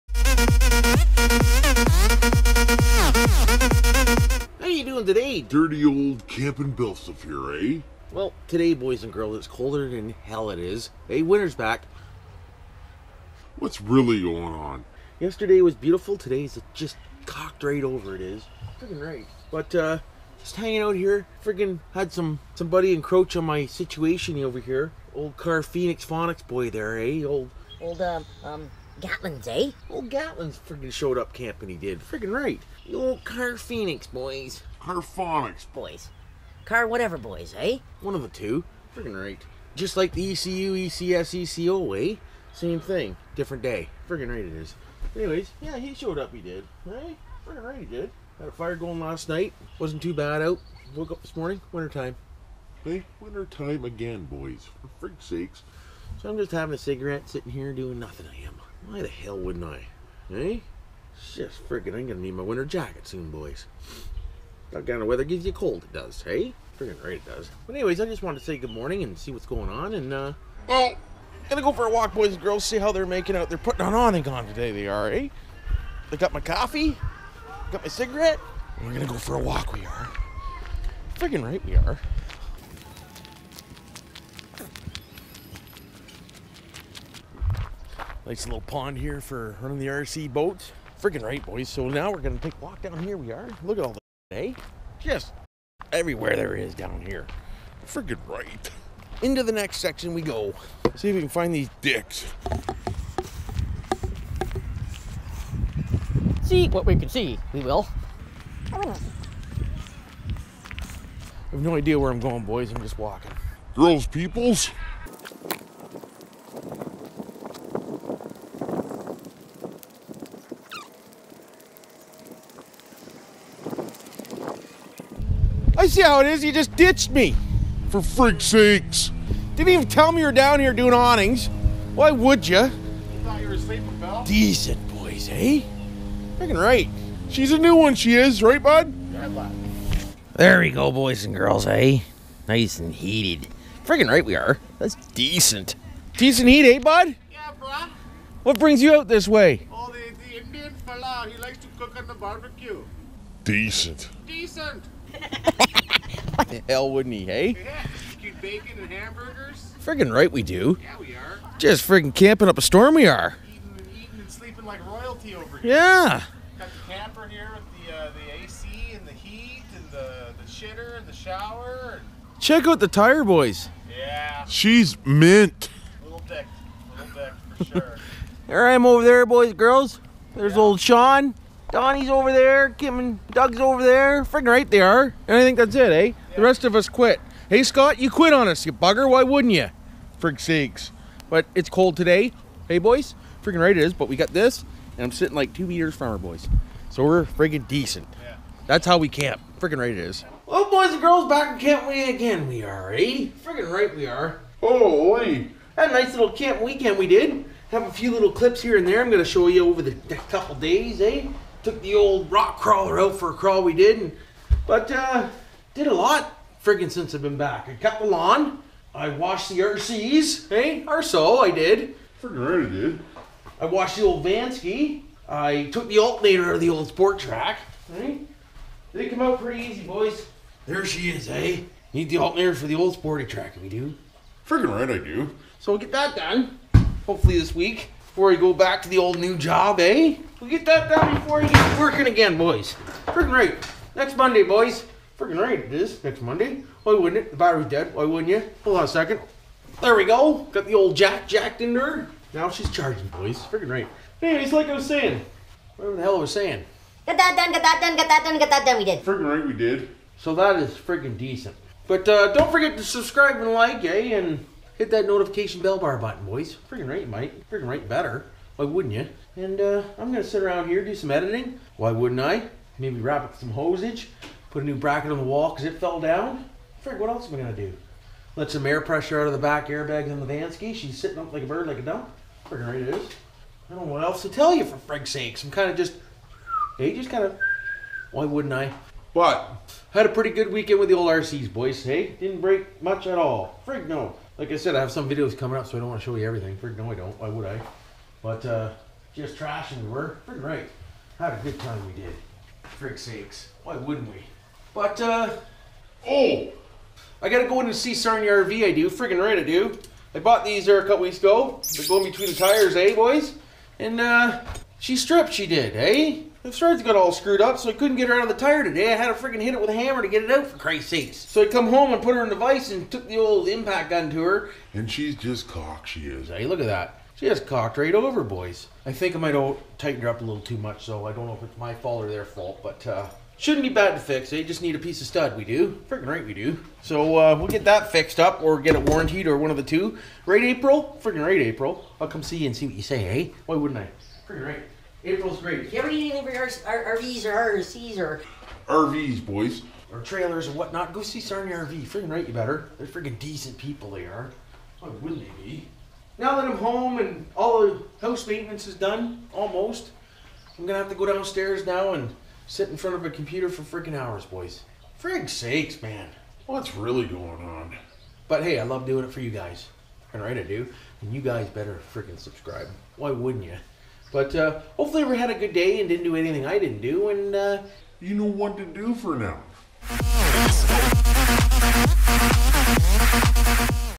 Camping with Car Phonix
Had a great time camping this weeks and with friends and one crazy guy .

Check out CarPhonix CarAudio

BillSiff
579 Main St
Wyoming Ont.
Canada
N0N 1T0